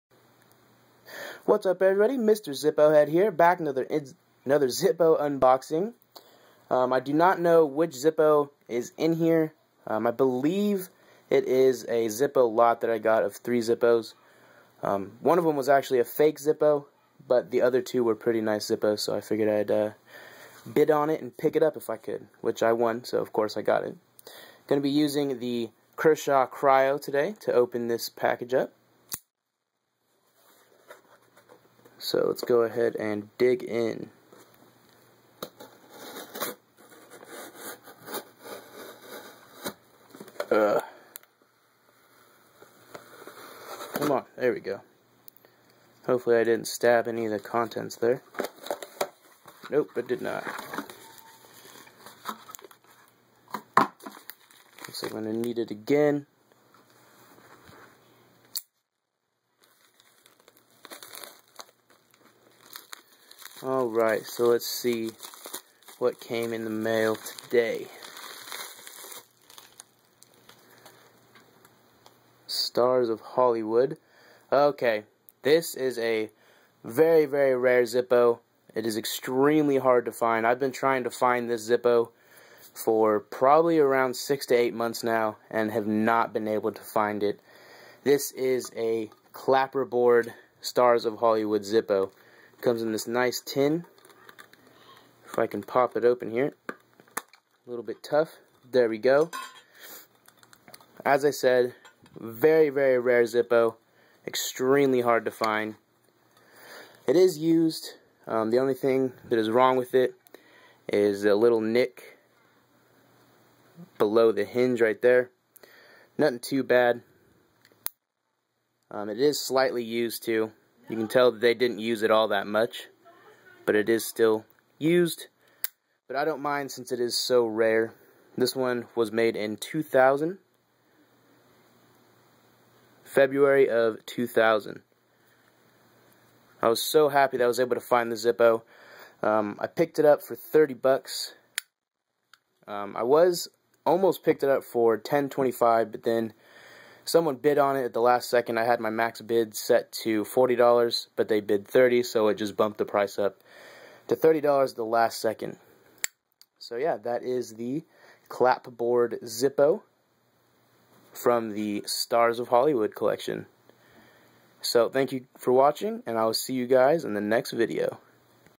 Zippo Unboxing - Stars Of Hollywood Clapboard
This is a very rare Zippo! I have been trying to find this Zippo for the past six months or so and I finally found it! :)